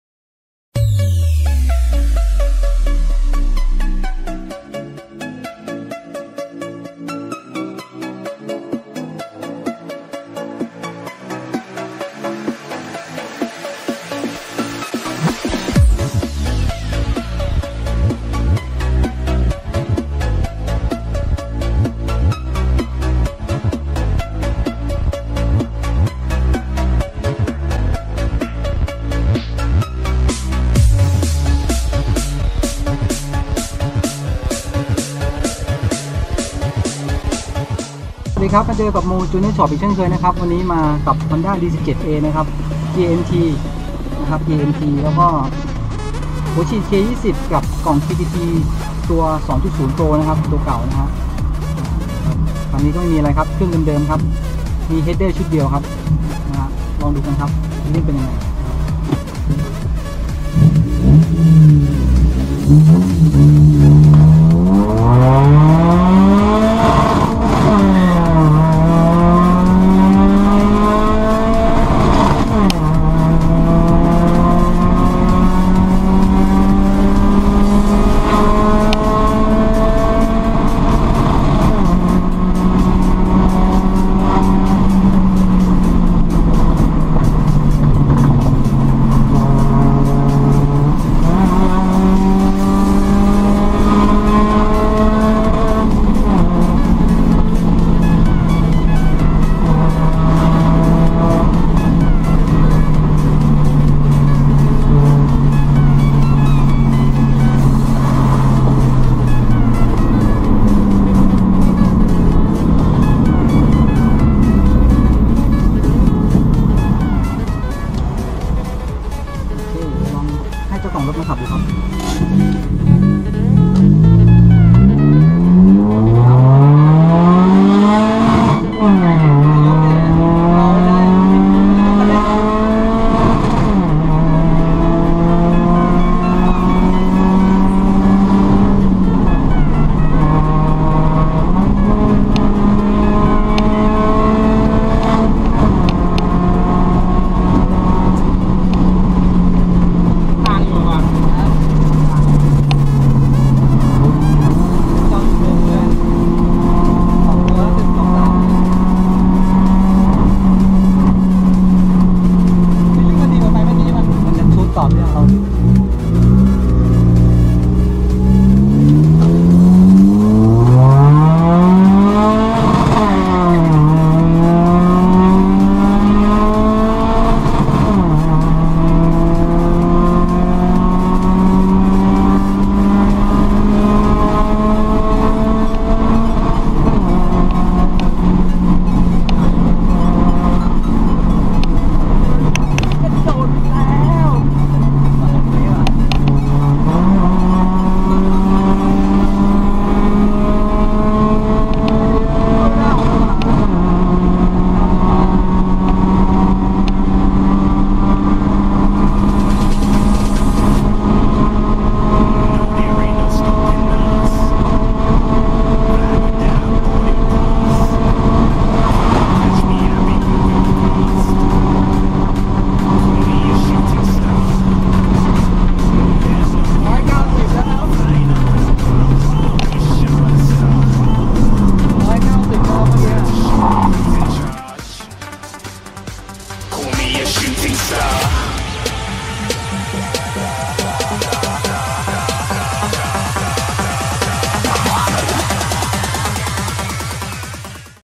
KKT V 2.0 Pro Tuner with Honda Civic ES 1.7 MT By Mo Tuner
💥ECU: KKT V 2.0 Pro Tuner (Piggy Back)
💥Model : Honda Civic ES
💥Engine : D17A MT
💥Injector : Honda K20A (290 cc)
💥Fuel : E85
💥Power : -

สนใจติดตั้ง หรือเป็นตัวแทนจำหน่าย ติดต่อ
🔹Page : KKT -  By Mo Tuner
🔹ID Line: MoTuner